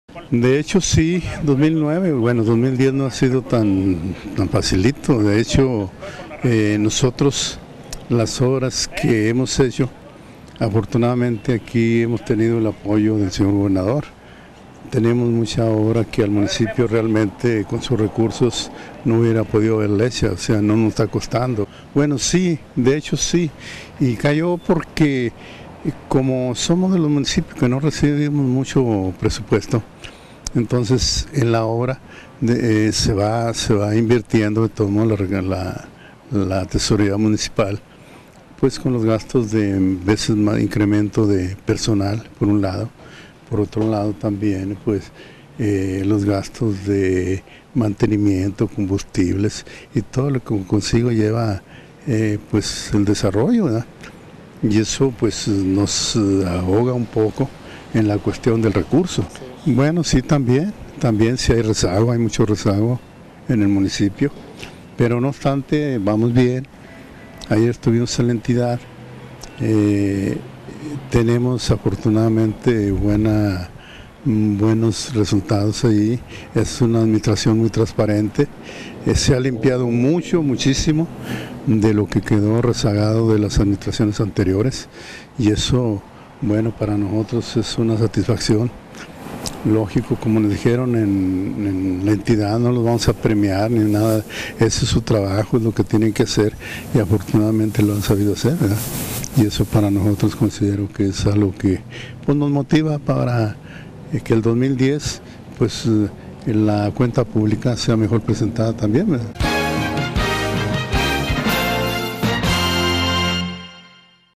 enrique garcia presidente vicente guerrero dgo municipal.mpg
Aumento de los combustibles en este año, situación que complico la mezcla de recursos para realizar obra.

3.- El año mas complicado para los actuales gobiernos municipales fue el 2009, sin embargo este 2010 Tampico ha sido nada sencillo, asevero el alcalde de Vicente Guerrero, Enrique García, quien manifestó que las finanzas propias fueron impactadas por el aumento de los combustibles en este año, situación que complico la mezcla de recursos con los otros niveles de gobierno para hacer obra.